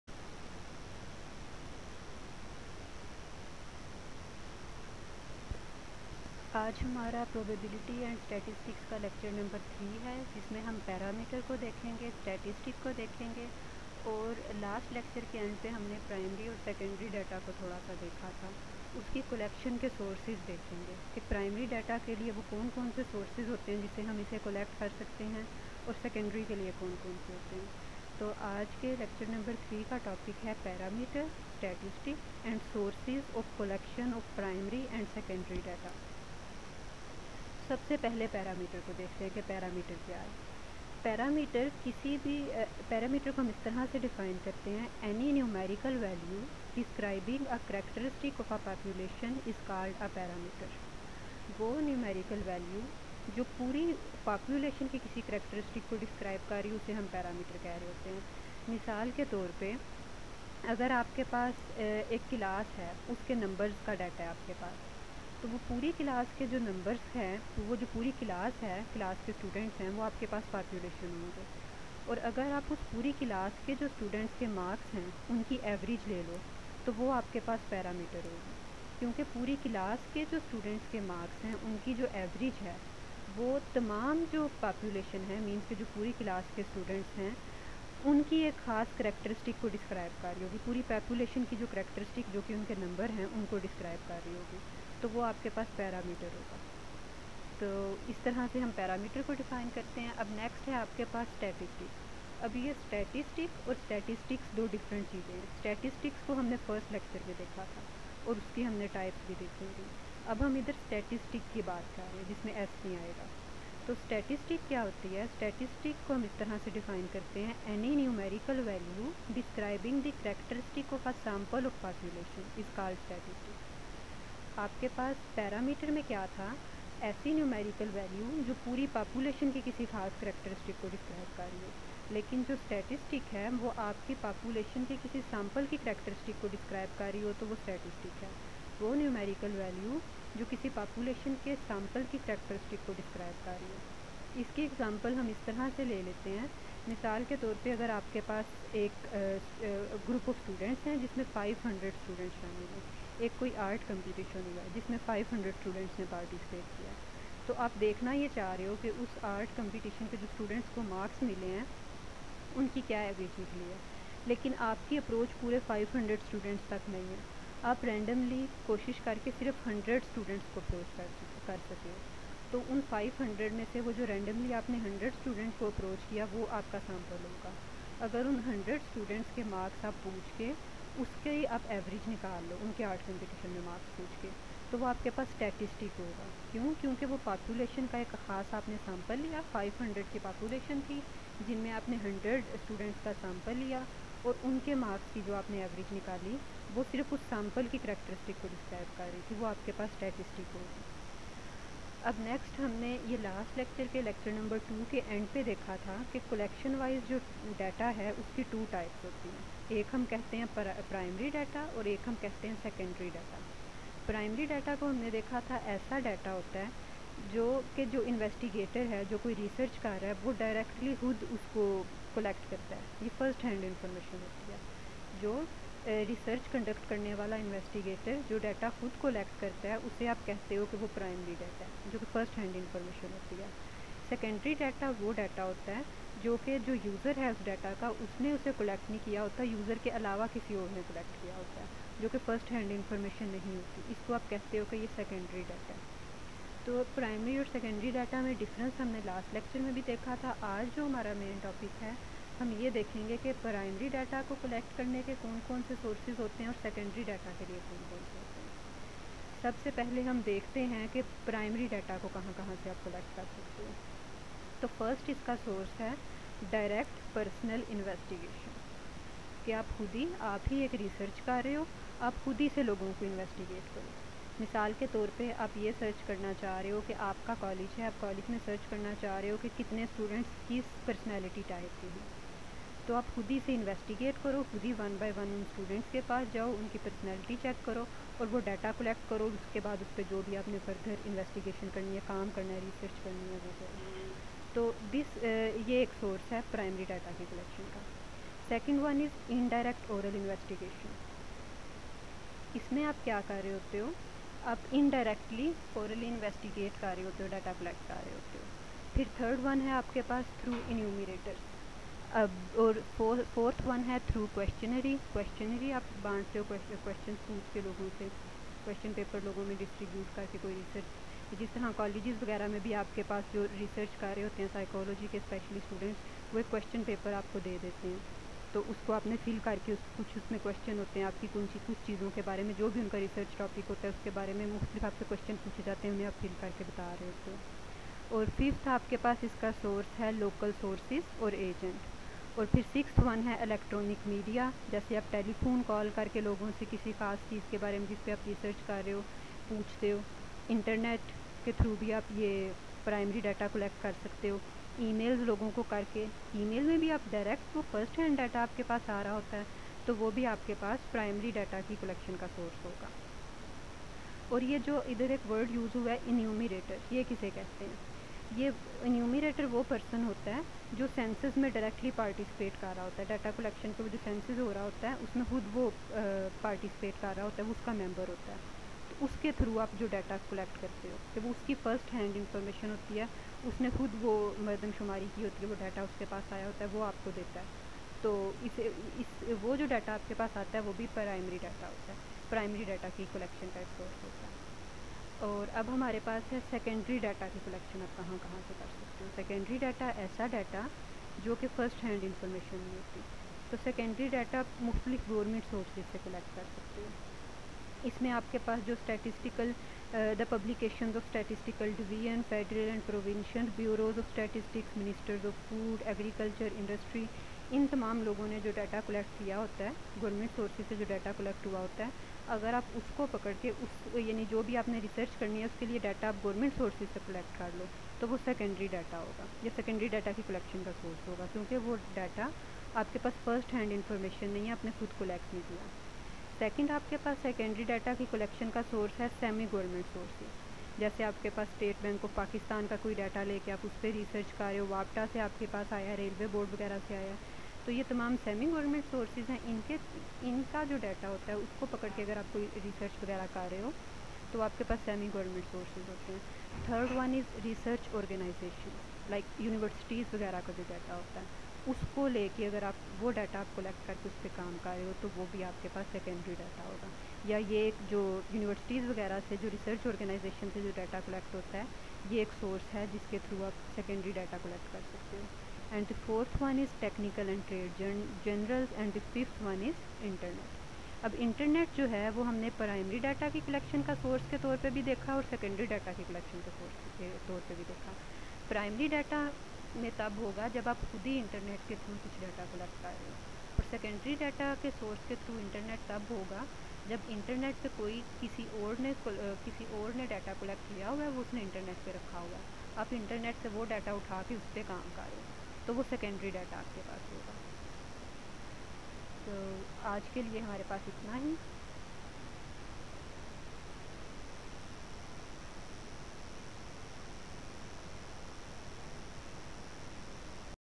statistic, parameter and sources of collection of primary and secondary data
probability and statistics lec 3